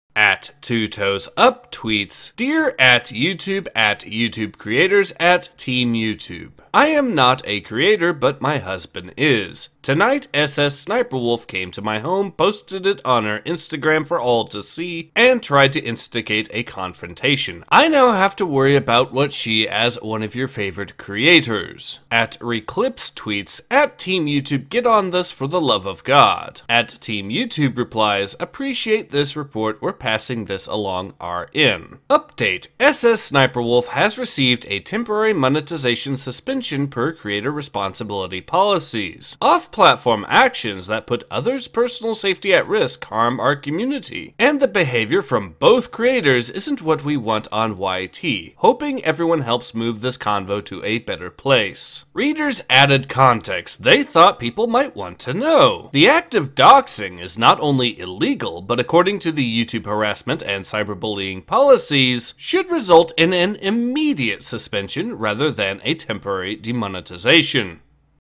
YouTube Gets The Community Note Treatment On Twitter Part 3
Today ACE shows an excellent example of Community Notes on Twitter once again calling out YouTube, this time with regards to them protecting SSSniperWolf by not following what their own Terms Of Service demand in terms of punishment for her actions.

Channel Donors:
Jake Lastname
Smokin and Jokin
Fernin

Stefan Paszki

CIARAN BYRNE

nathanx2000
[soopy] cartoons
Matt DieBold